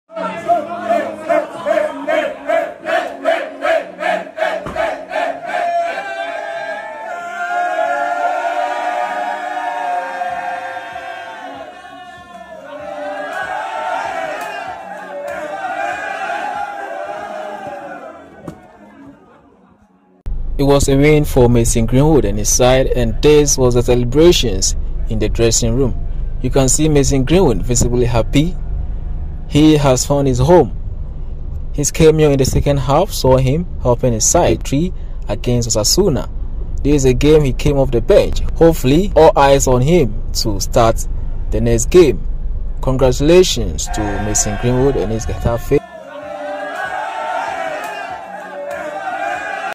SCENES!! Mason Greenwood leads Getafe dressing room celebrations after impressive debutover Osasuna
Mason Greenwood Vs Osasuna
Getafe Vs Osasuna
Mason Greenwood debut